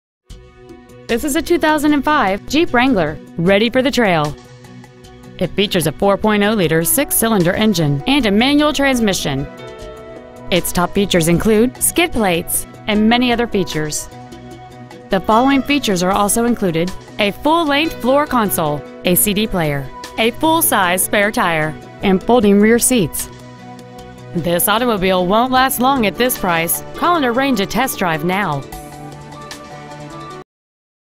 Preowned 2005 Jeep Wrangler Phoenix AZ
We've been honored to serve the Phoenix AZ area , we promise that your experience at our dealership will exceed your expectations !

 Year : 2005
 Make : Jeep
 Model : Wrangler
 Engine : 4.0L SMPI I6 POWER TECH ENGINE
 Trans . : Manual
 Exterior : Other
 Miles : 136,936
 Interior : Other